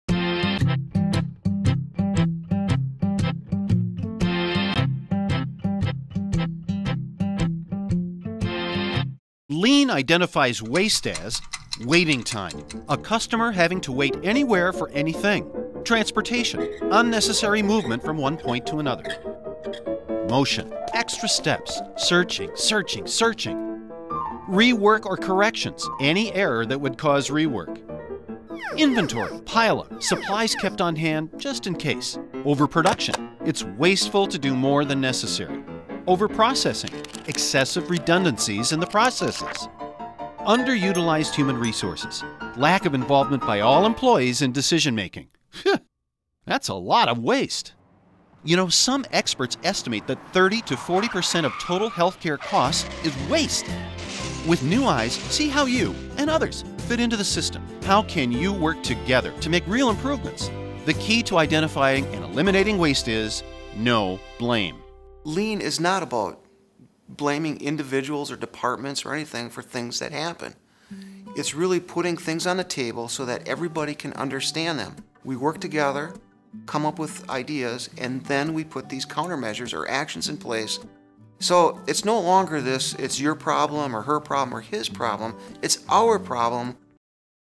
Optima Trailer: Lean: A Healthcare Remedy
Most healthcare organizations, as part of their mission statements, strive to ensure high quality, safety, effectiveness, and patient-centered care through continual improvement. Healthcare organizations, just like their patients, are always trying to get better.

As a strategic partner, Optima will work to ensure your social, economic, environmental, and cultural processes are always improving. We can help your healthcare organization stay aligned and focused on waste elimination strategies that drive sustainability and economic success. We will focus on your building and grounds, operational and administrative processes, environmental, safety and transportation risks and community impact to help achieve sustainable growth within the healthcare industry.